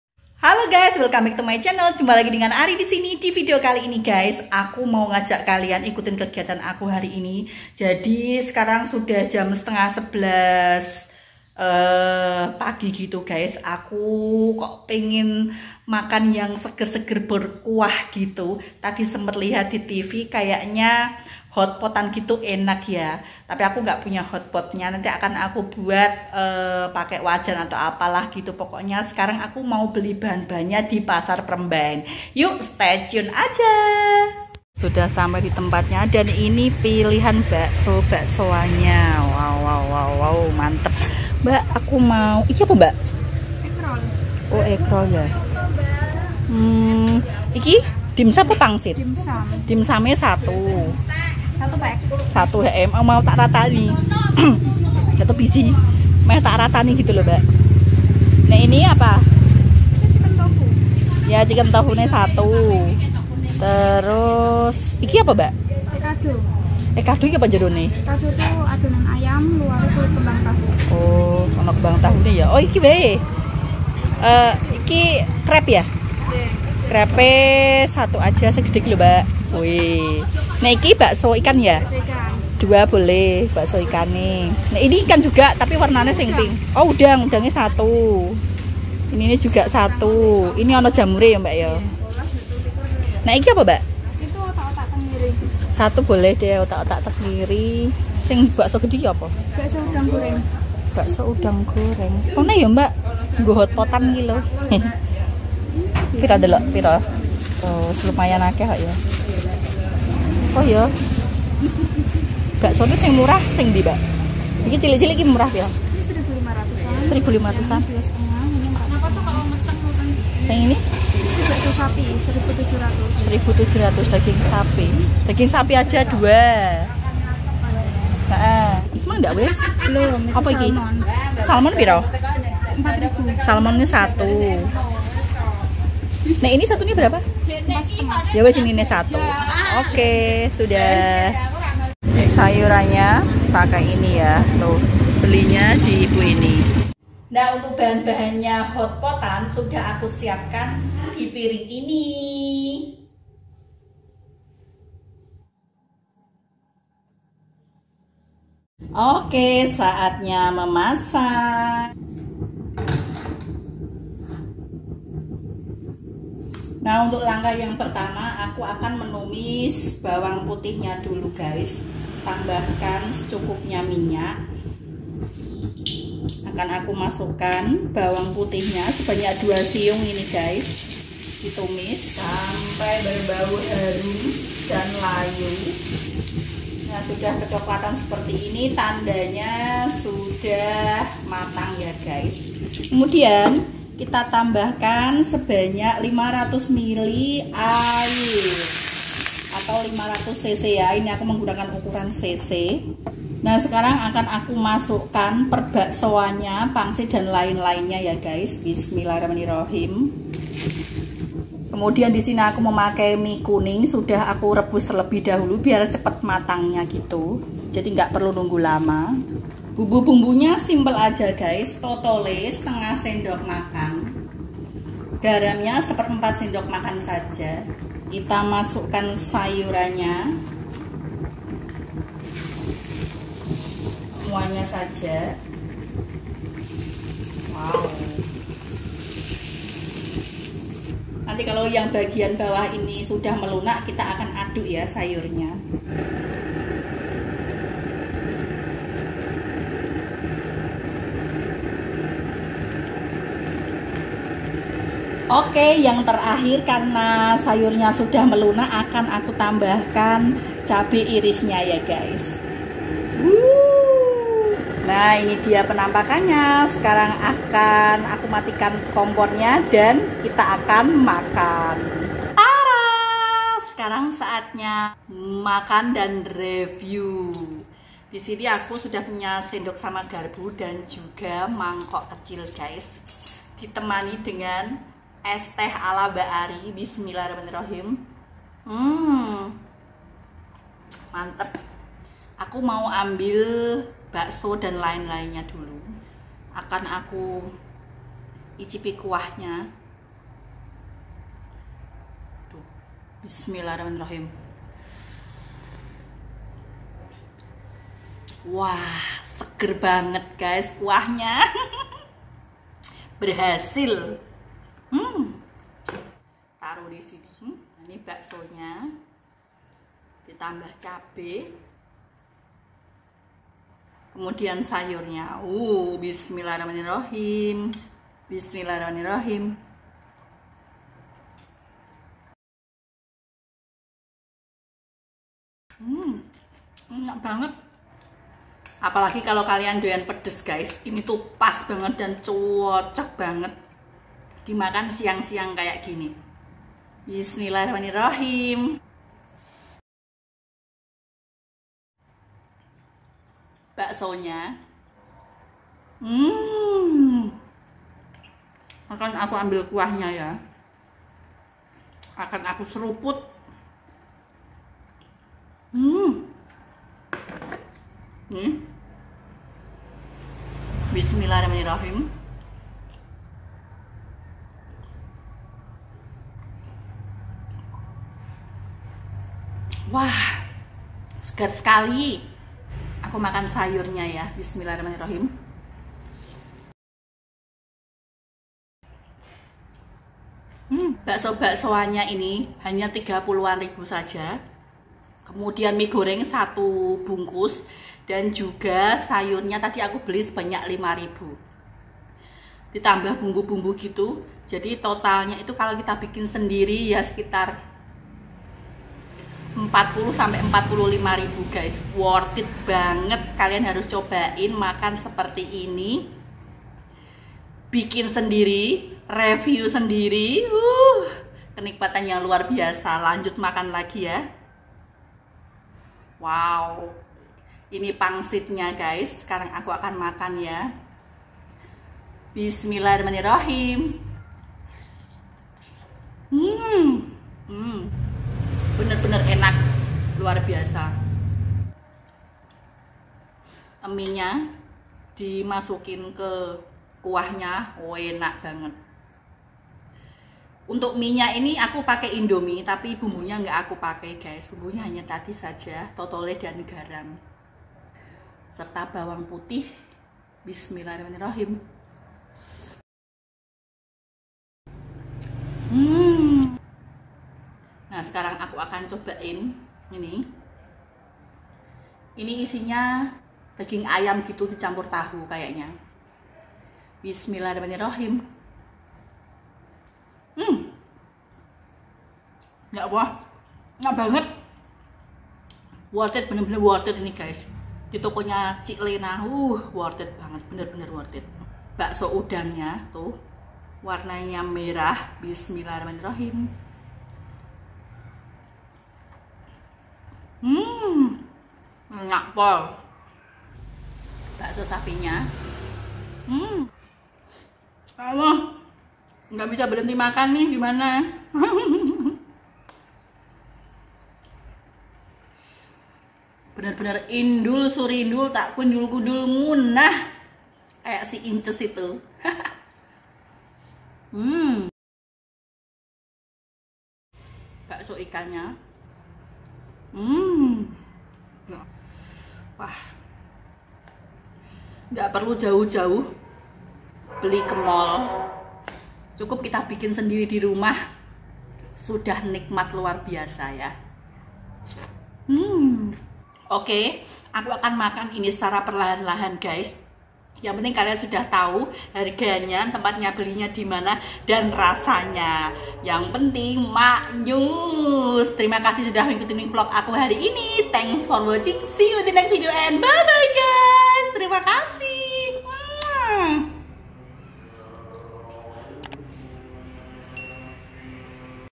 HOT POT RECIPE || MASAK PAKAI WAJAN TAMBAHKAN BUMBU SEDERHANA BAWANG PUTIH TOTOLE GARAM AUTO MUKBANG
halo guys simak video aku ya
terimakasih

Halo sahabat onlineku vlog aku hari ini adalah ingin tunjukkan ke kalian bagaimana cara memasak hot pot, resep dan dimana aku membeli bahan bahan pembuatan hot pot. Jadi untuk sayur dan olahan bakso baksoan aku membelinya di pasar prembaen semarang. Dan yang istimewa adalah bumbu yang aku gunakan cukup bawang putih totole garam dan jika ingin pedas tamvahkan cabe rawit. So stay tune